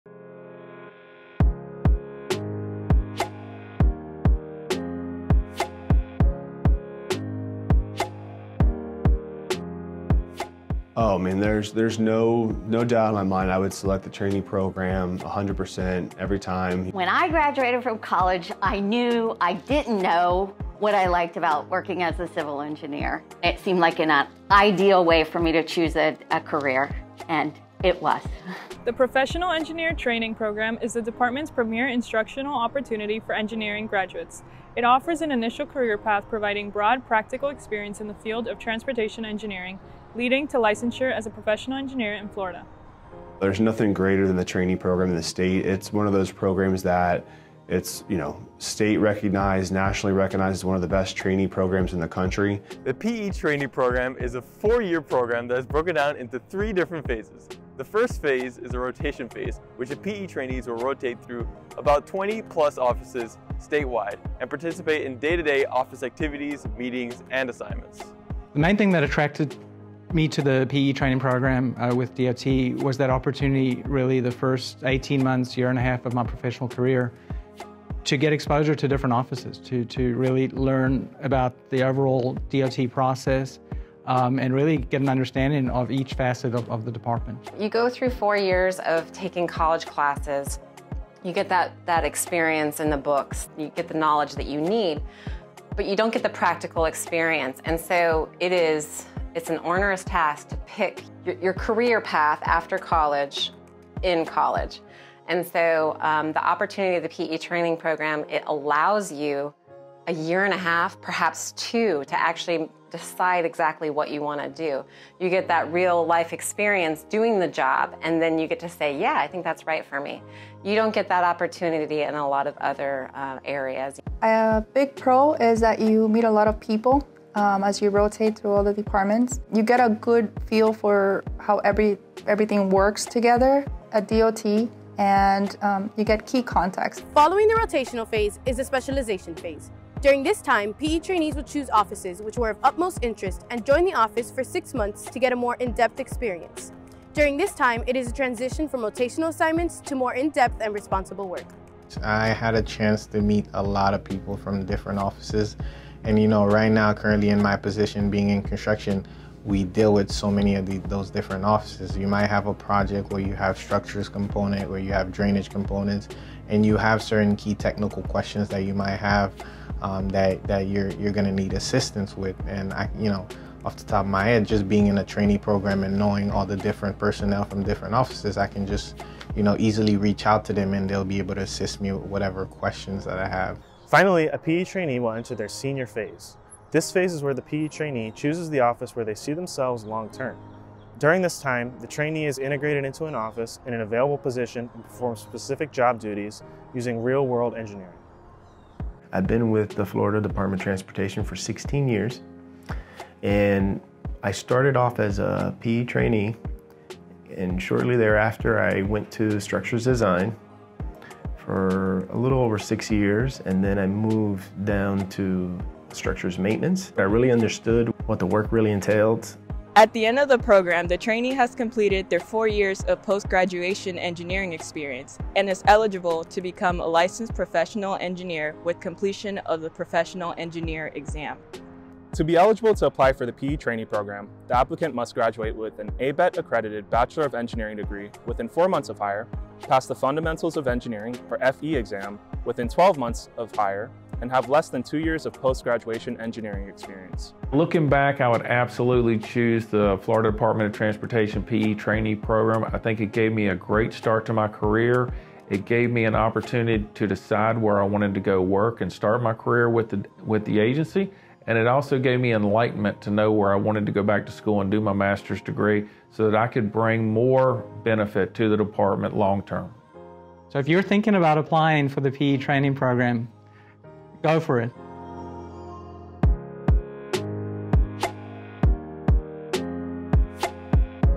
Professional Engineer Trainee Program
The Professional Engineer (P.E.) Training Program is the Department's premier instructional opportunity for engineering graduates by offering an initial career path providing broad and practical experience in the field of transportation engineering, leading to licensure as a Professional Engineer in Florida.

This 4-year training program encompasses all aspects of the Department's operations and prepares the trainee for a role in organizational leadership.